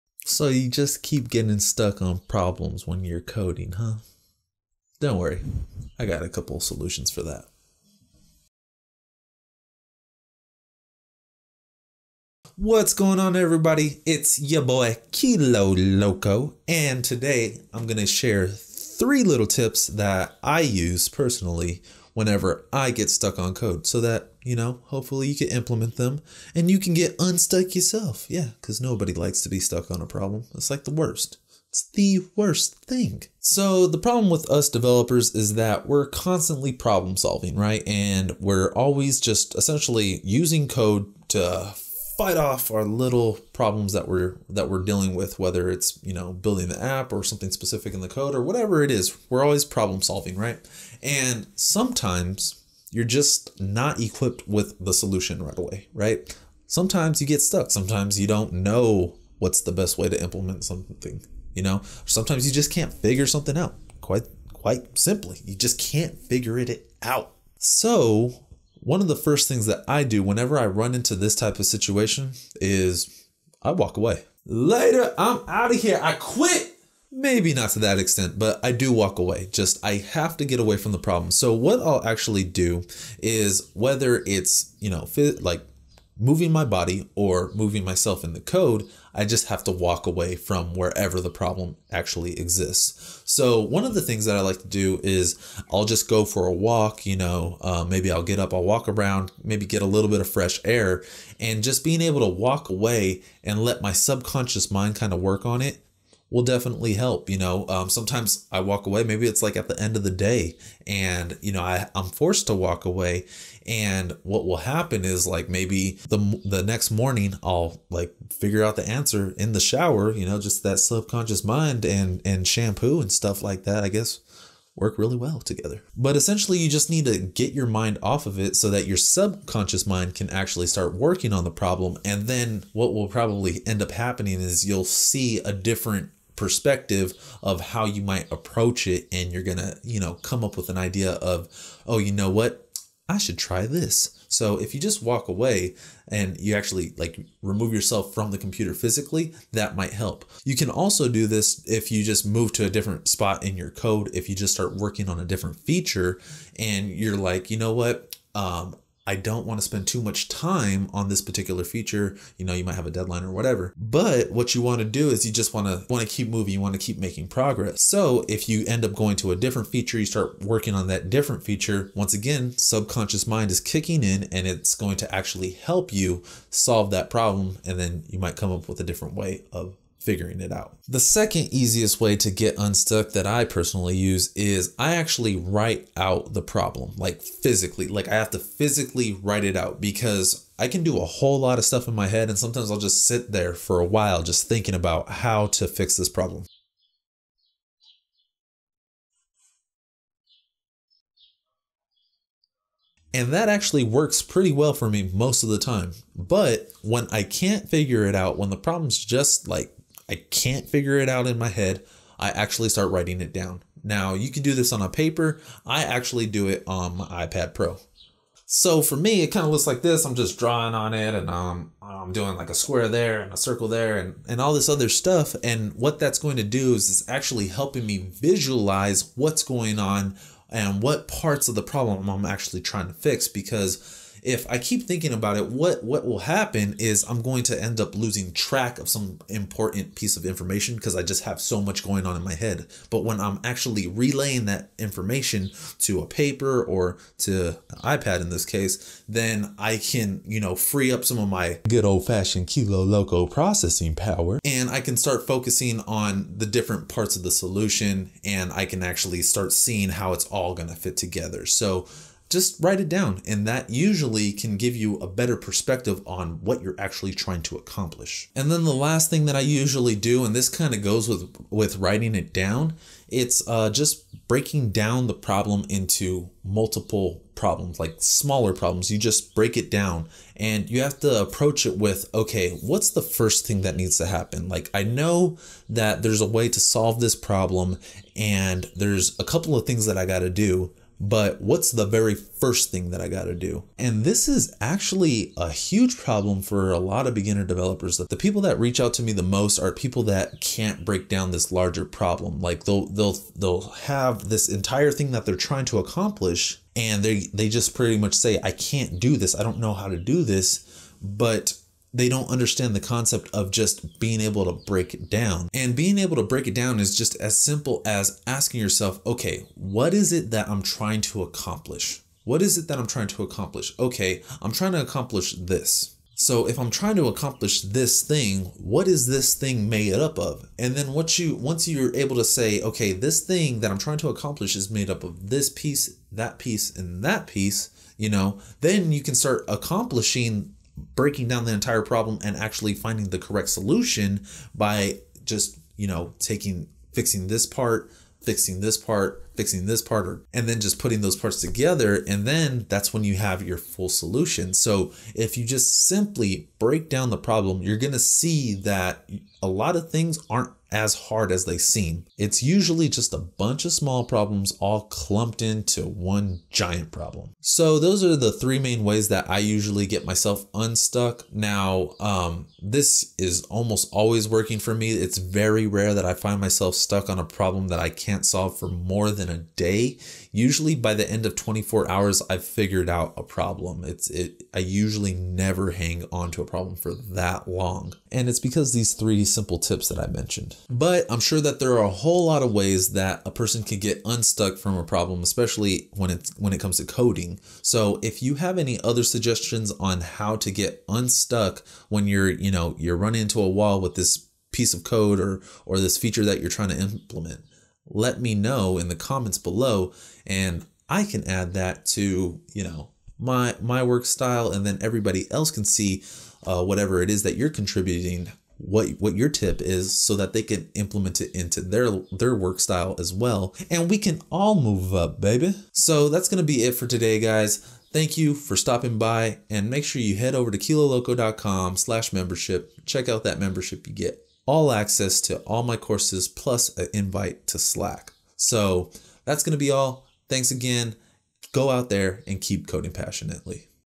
How To Get Unstuck While Coding
Today Kilo Loco goes over how to get unstuck when you run into a problem while coding. Kilo has 3 tips that will help you get over your problems and keep moving forwards as an efficient developer.

GEAR

Code Passionately T-Shirt

Laptop - 2017 MacBook Pro